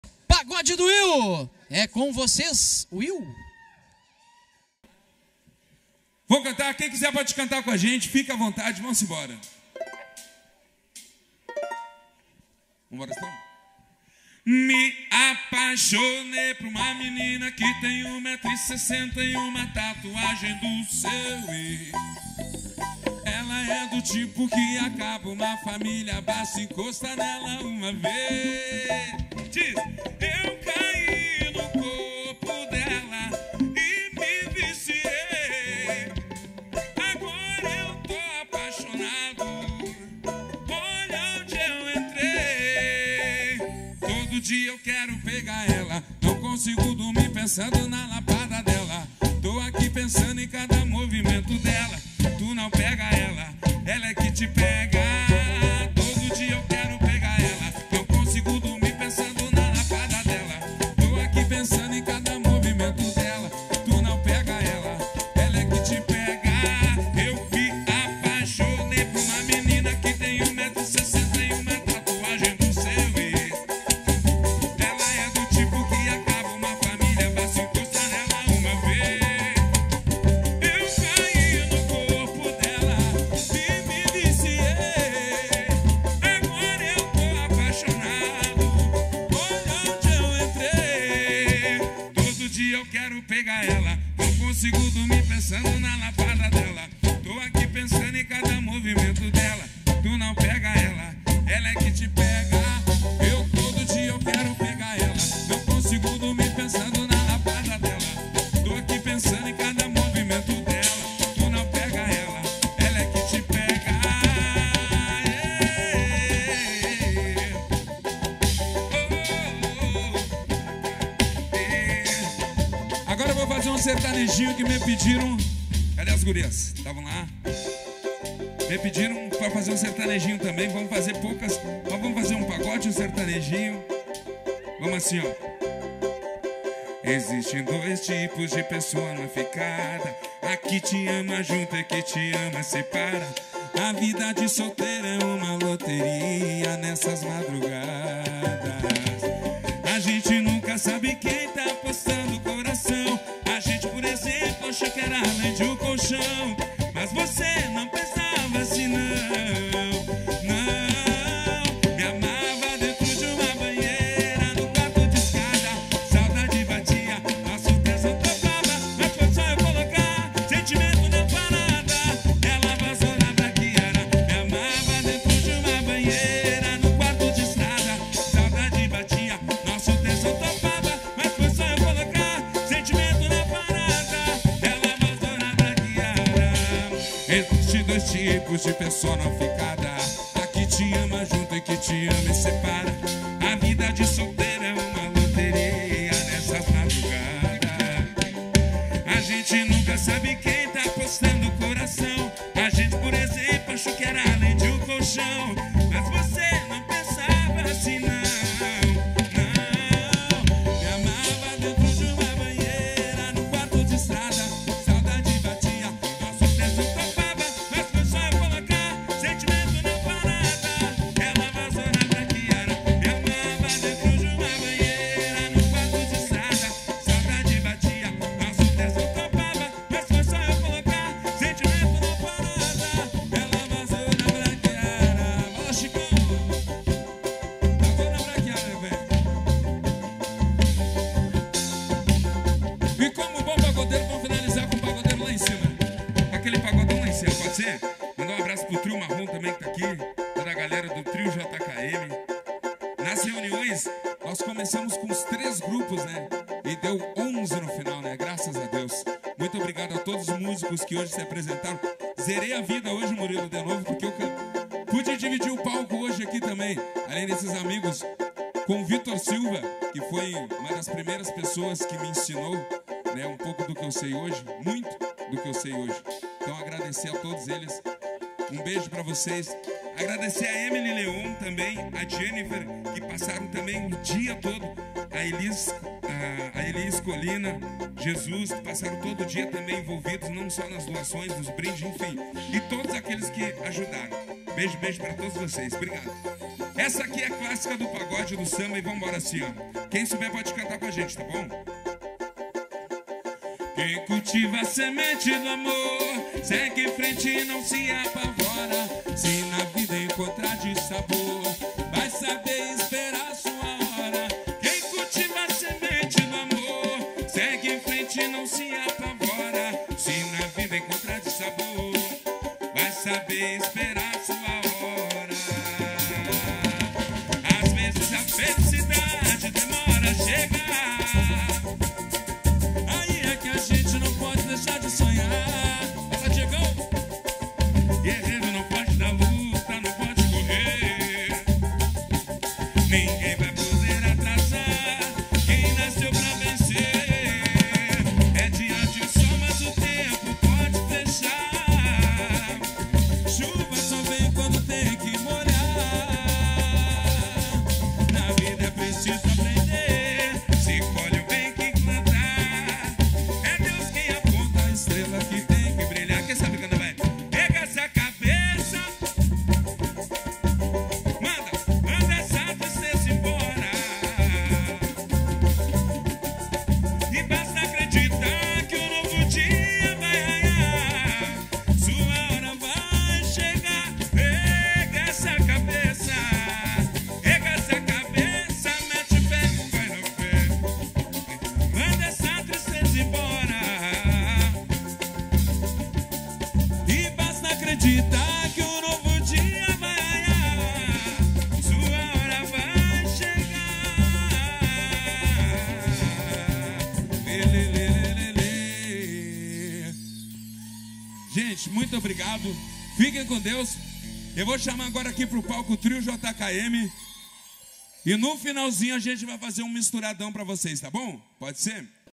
PAGODE DO WILL | AO VIVO | BALANÇO SOLIDÁRIO | DOM PEDRITO/RS
🎶 Reviva conosco os melhores momentos do show do Pagode do Will em Dom Pedrito, RS, durante o evento Balanço Solidário.

📍 O CTG Herança Paternal esteve de portas abertas para receber ma noite de música e solidariedade, onde arrecadamos doações de produtos essenciais para as vítimas das enchentes no Rio Grande do Sul. Juntos, fazemos a diferença!

📡  REALIZAÇÃO:
MOON Produtora | TV Web Publicidades | Rede Conesul | Daniel Films | V.S Som e Luz.